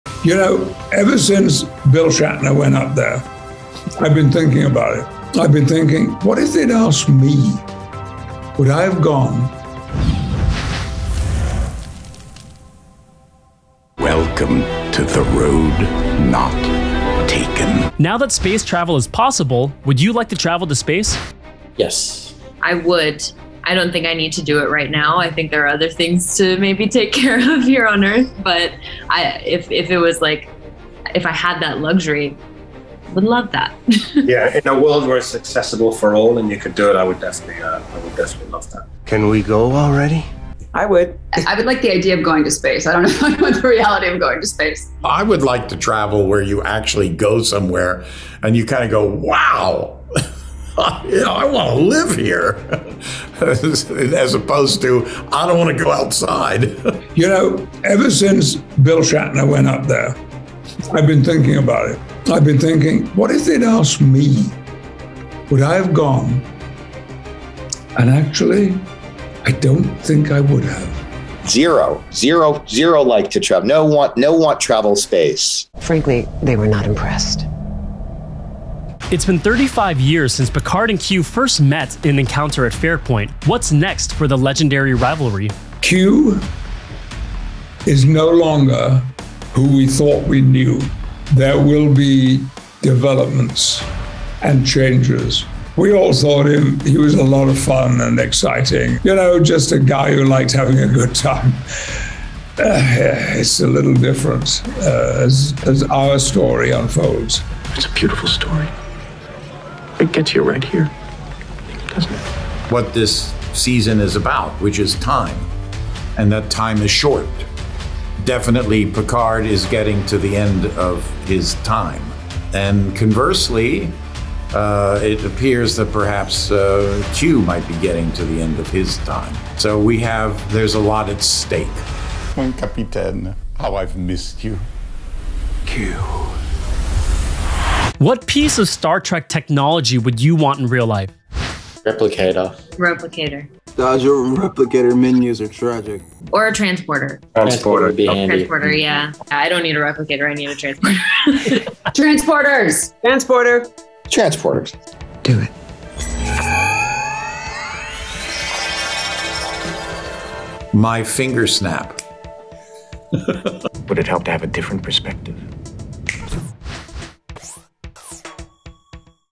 Burning Questions With Sir Patrick Stewart & the Cast of "Picard"
Would the cast of Picard actually go to space if given the chance? Stars SirPatrickStewart, JohnDeLancie, and JeriRyan answer—and their responses may surprise you. 🚀

Star Trek: Picard
Follow-up series to Star Trek: The Next Generation (1987) and Star Trek: Nemesis (2002) that centers on Jean-Luc Picard (Sir Patrick Stewart) in the next chapter of his life.